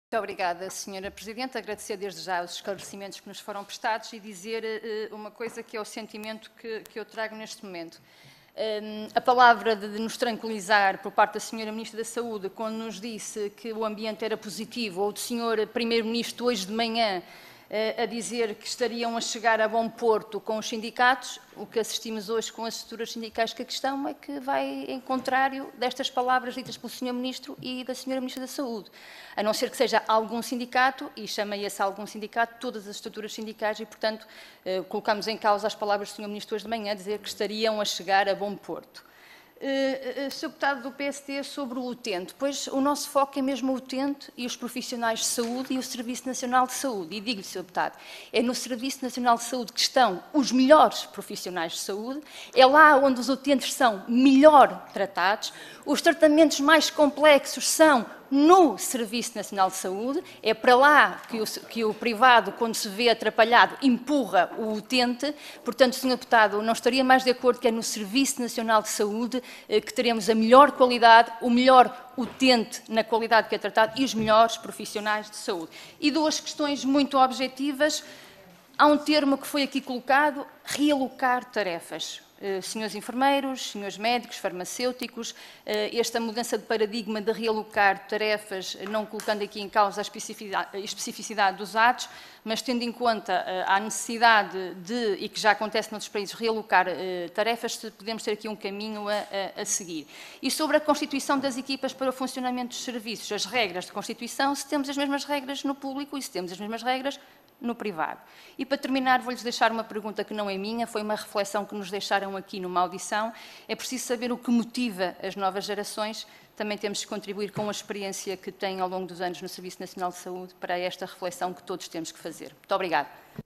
17-07-2024 | Audição conjunta: FNAM – SEP – STSS - FNSTFPS | Susana Correia 2ª
XVI Legislatura
Intervenção da deputada Susana Correia - Audição conjunta da Federação Nacional dos Médicos, Sindicato dos Enfermeiros Portugueses, Sindicato Nacional dos Técnicos Superiores de Saúde das Áreas de Diagnóstico e Terapêutica e Federação Nacional dos Sindicatos dos Trabalhadores em Funções Públicas e Sociais sobre as dificuldades de funcionamento dos serviços de urgência (requerimento do PCP) - Comissão de Saúde